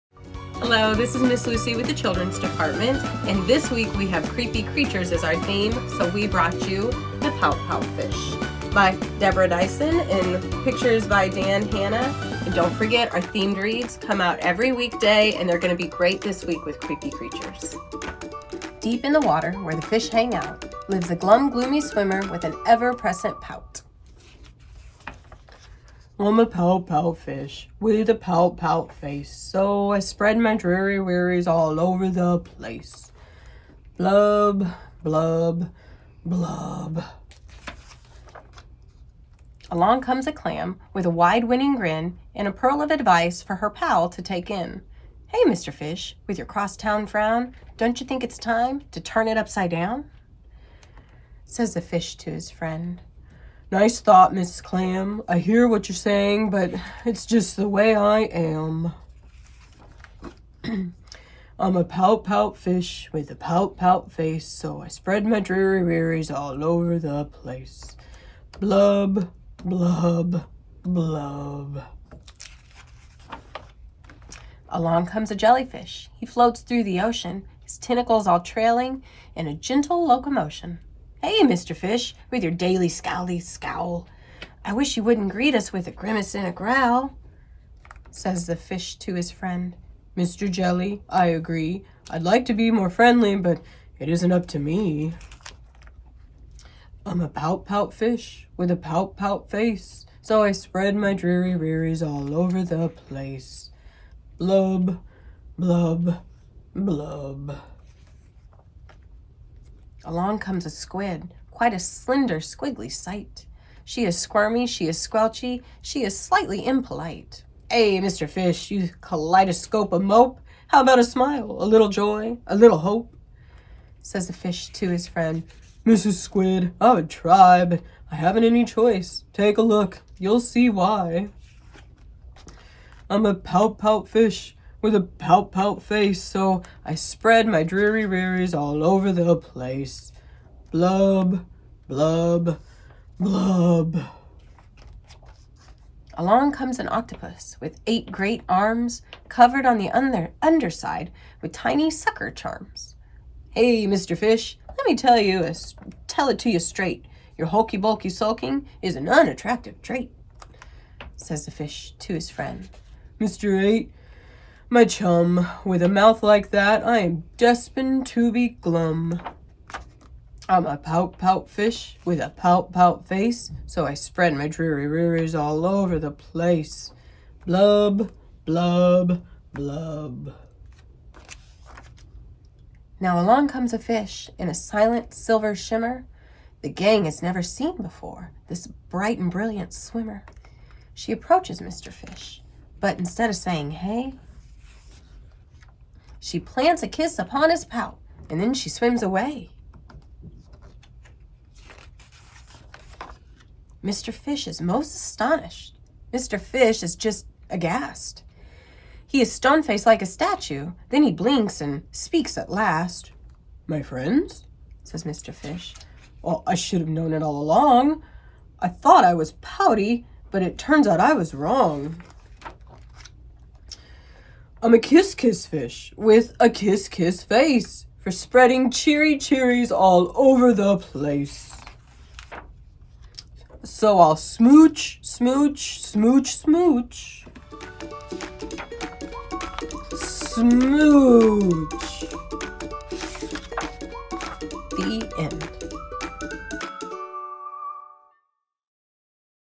Read Aloud: The Pout-Pout Fish by Deborah Diesen
Read all of the Summer Reading Program's books with us for a chance at winning the grand prize at the end of the summer! Go to our website for a calendar view of our readings and events